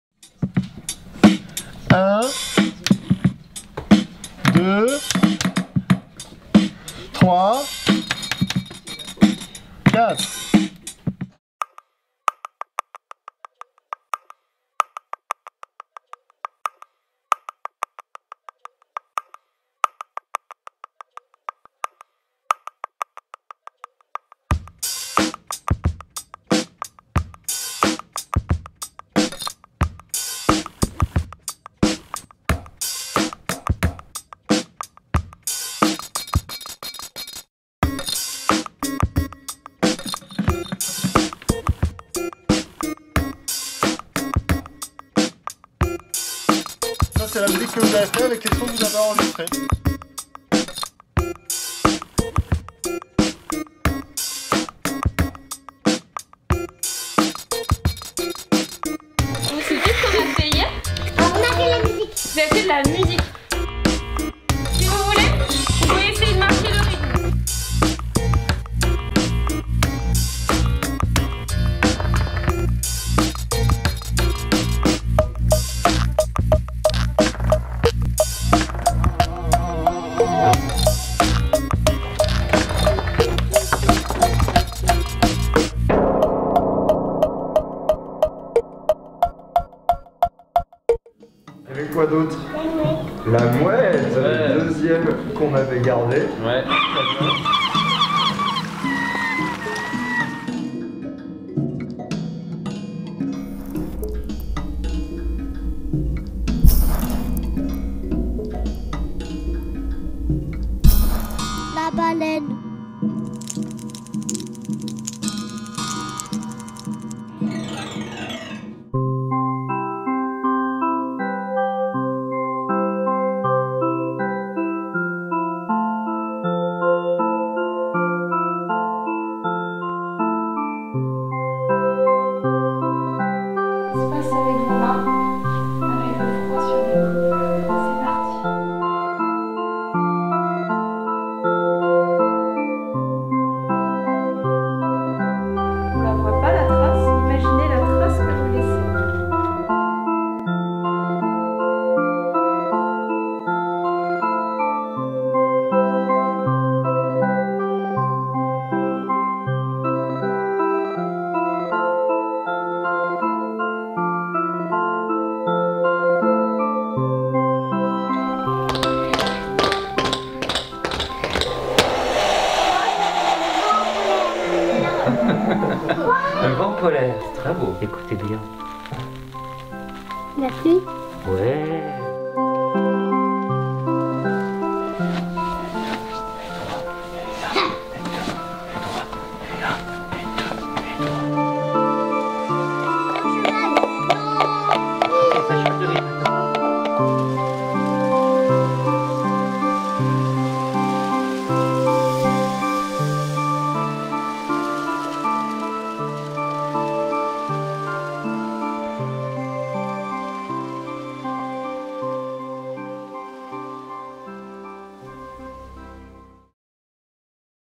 Petits artistes pour grand projet : réenchanter Cleunay
Éducation Artistique et Culturelle - Saison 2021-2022

Pour la saison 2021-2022, l’école maternelle Champion de Cicé a fait appel à l’Antipode et au Théatre National de Bretagne pour imaginer un projet de proximité croisant les artistes et les disciplines auprès d’enfants de 3 à 6 ans et de leurs familles.

Au total, sept artistes ont été répartis dans trois classes de maternelle autour des disciplines : musique, danse, théâtre et arts plastiques. Un projet fort à découvrir en vidéo avec le reportage de Benoist Lhuillery et en photos avec le reportage de Gwendal Le Flem.

Vidéo : Benoist Lhuillery